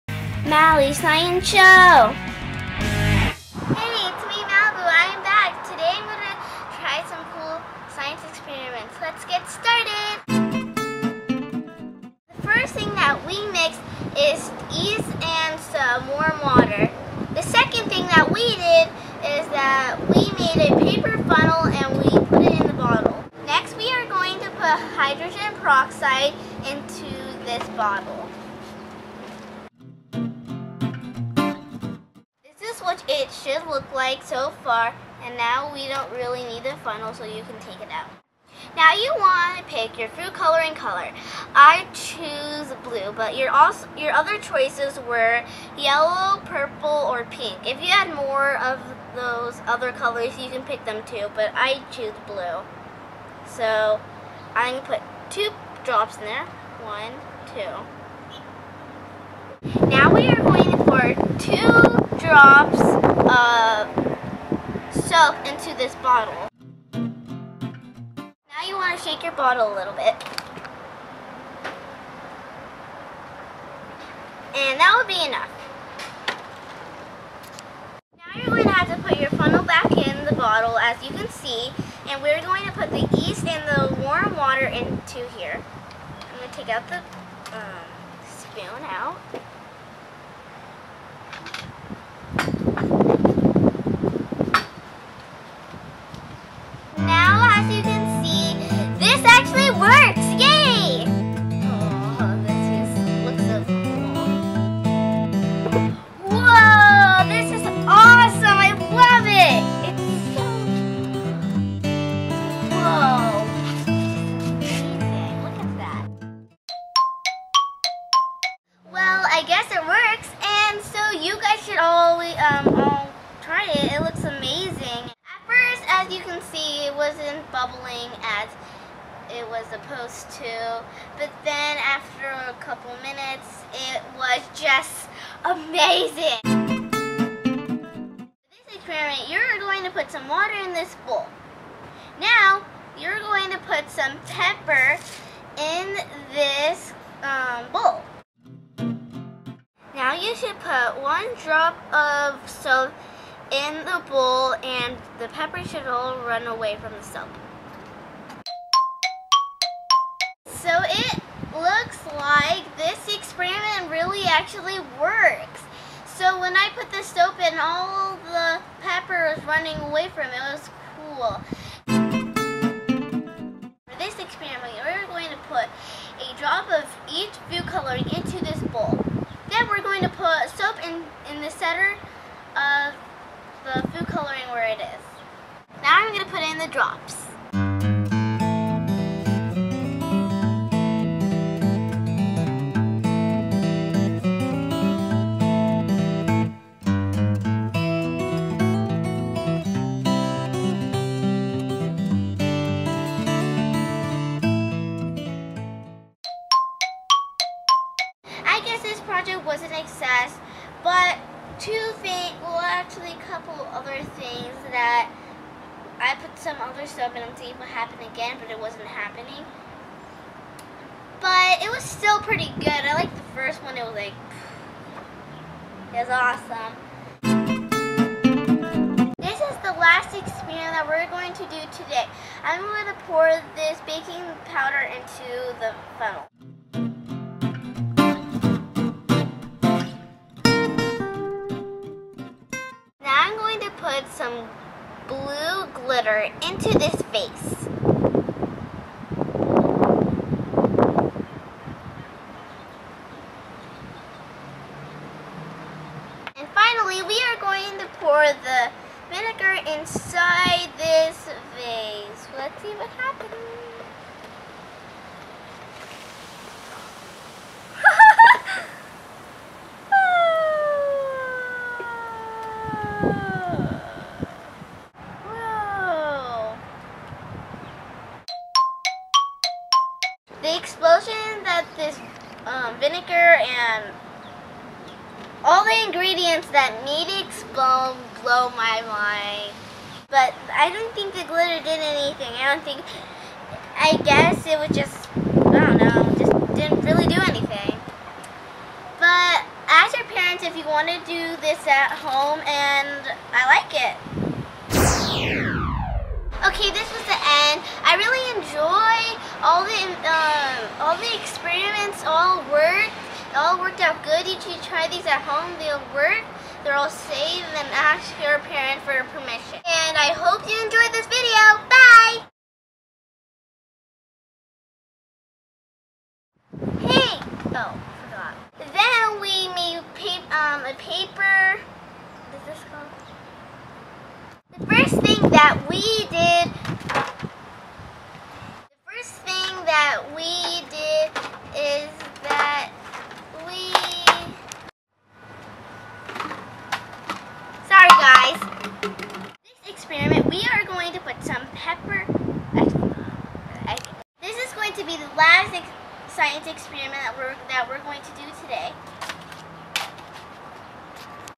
Cool Science Experiments
Hey Everyone, I'm back and trying out some cool experiments. I'm so glad all of them worked you guys should try some of these out. If you guys like the "Mali's Science Show" tell me and we will continue to do more of them.